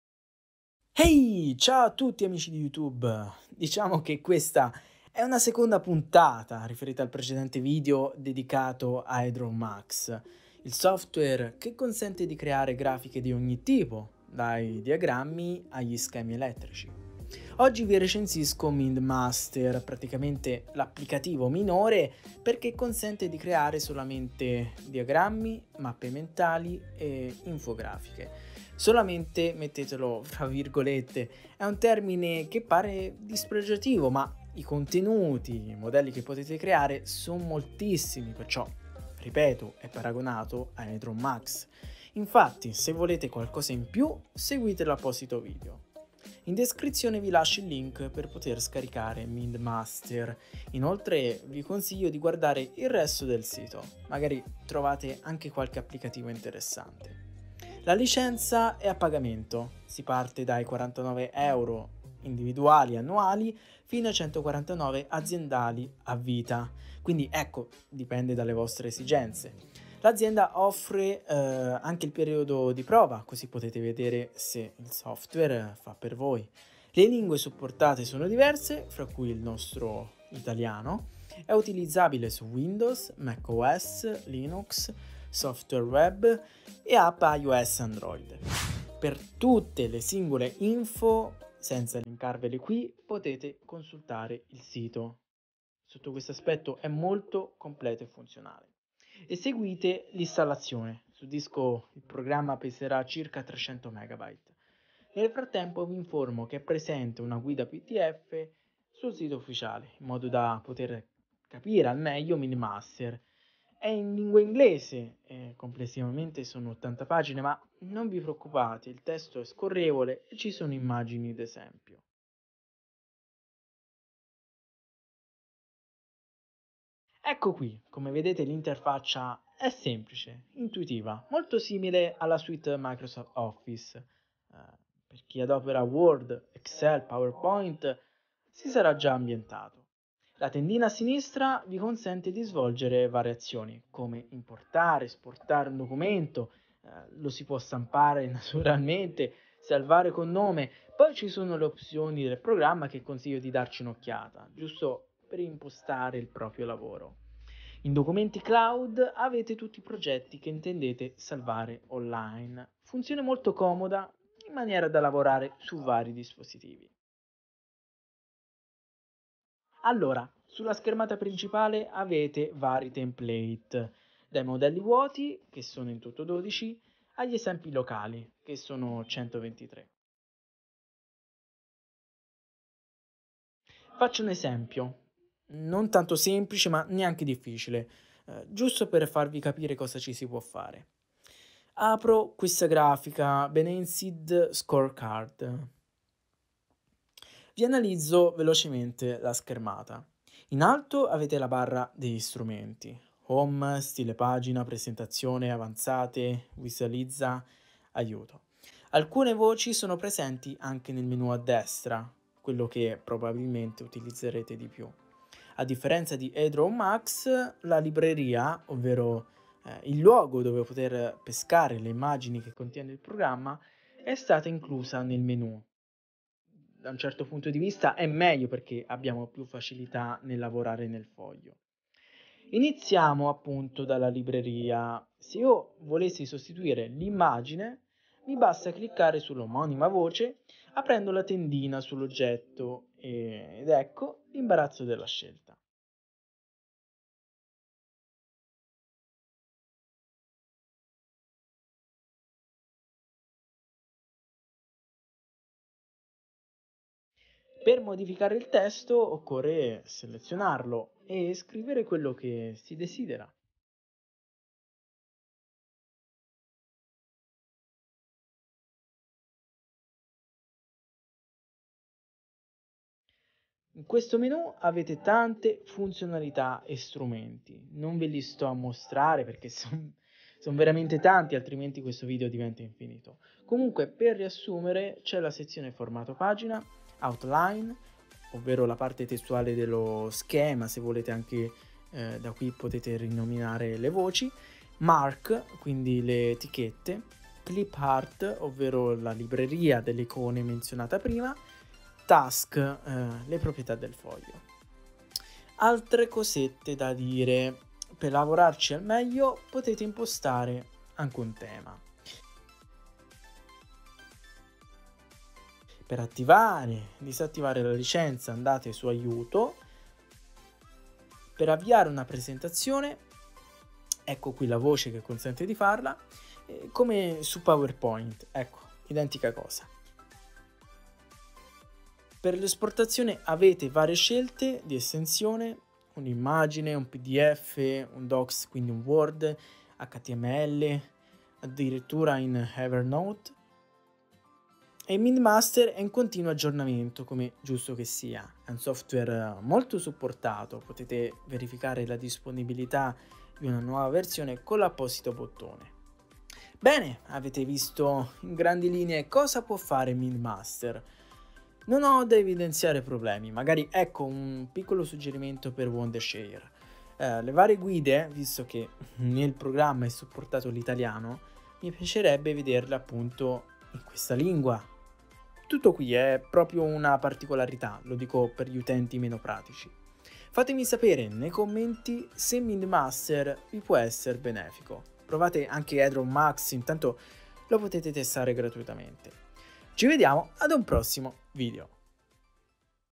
Wondershare MindMaster: progettare infografiche, diagrammi, mappe mentali e altro
*COMPENDIO*
In questo secondo appuntamento, riferendosi al video precedente presento il programma MindMaster, sviluppato dalla nota azienda Wondershare.
In sostanza, è l'applicativo minore di EdrawMax poiché consente solamente di creare diagrammi, mappe mentali e infografiche. "Solamente" è un termine che pare dispregiativo, ma i modelli disponibili sono molti.

Disponibilità, prezzi, download e installazione
Nella sezione "LINK E CONTATTI" di questa descrizione è possibile scaricare MindMaster, suggerendovi di visualizzare altre pagine del sito web ufficiale siccome sono in catalogo diversi altri applicativi che potrebbero risultare interessanti.
La licenza è a pagamento, si parte dai 49€ in forma individuale/annuale fino ai 149€ per le aziende con durata permanente. L'azienda offre una versione di prova, così da comprendere le funzionalità presenti. Le lingue supportate sono diverse fra cui l'italiano. È utilizzabile su Windows, macOS, Linux, software-web, applicazione iOS e Android.
Su Windows MindMaster occupa in memoria circa 300 MB; la versione provata è la 8.0 dove sono stati risolti diversi bug. L'installazione è breve e intuitiva: viene creata solo la cartella contenente i file necessari pel funzionamento.
Sul sito ufficiale sono presenti varie guide, sia visive (sotto forma di video) che testuali (PDF di 80 pagine, solamente in lingua inglese). Sempre sul sito si trova una sezione dedicata al profilo personale, in modo da controllare le proprie licenze in possesso, chiedere supporto ai tecnici e riscattare un codice regalo per poter usufruire dei software.

Schermata principale e di progetto
L'interfaccia è semplice e intuitiva, molto simile al pacchetto Microsoft Office (per chi adopera Word, Excel, PowerPoint sarà già in un ambiente familiare).
Il menu di sinistra consente di svolgere varie azioni, come importare, esportare, stampare, salvare con nome un documento. Presenti anche le opzioni del programma in modo da impostare MindMaster secondo le proprie esigenze.
In Documenti cloud si hanno tutti i progetti che si intendono salvare online, funzione molto comoda in maniera da lavorare su vari dispositivi.
Sulla schermata principale ci sono i pre-impostati, altrimenti si può partire da un foglio vuoto.
Analizzo velocemente la schermata del progetto aperto. In alto si ha la barra degli strumenti che contiene le sezioni Home, Stile Pagina, Presentazione, Avanzate, Visualizza, Aiuto. Alcune di queste voci sono presenti anche nel menu a destra (quello che probabilmente si utilizza di più). Quest'ultimo presenta cinque sezioni: Formato Pagina, OutLine (la parte testuale dello schema), Mark (le etichette), Clipart (la libreria delle icone), Task (le proprietà del foglio).
A differenza di Edraw Max, la Libreria, ovvero il luogo dove poter pescare le immagini che contiene il programma, è stata inclusa nel menu. Da un certo punto di vista è meglio perché il programma diventa meno dispersivo.

Azioni sul foglio
Aggiunta o sostituzione di un'immagine: aprire, col tasto destro del mouse, il menu contestuale con l'omonima voce e dopodiché si attiverà la libreria.
Per modificare il testo occorre selezionarlo (due clic oppure Ctrl+A) e scrivere quello che si desidera, come accade del resto in molti programmi.
Dal menu a destra menzionato prima, si può accedere a varie personalizzazioni: dimensione, colore, carattere, sfondo, tema, effetto ecc.

Altri aspetti
• È possibile impostare un tema (icona Maglietta in alto a destra)
• Per attivare/disattivare la licenza andare sulla voce Aiuto (barra degli strumenti)
• Per avviare una presentazione (come su PowerPoint), andare nella barra degli strumenti e aprire la voce Presentazione.
• Si può verificare la disponibilità di una nuova versione aprendo la voce Aiuto.

*DATI VIDEO*
• Programma di registrazione e montaggio: TechSmith Camtasia 2019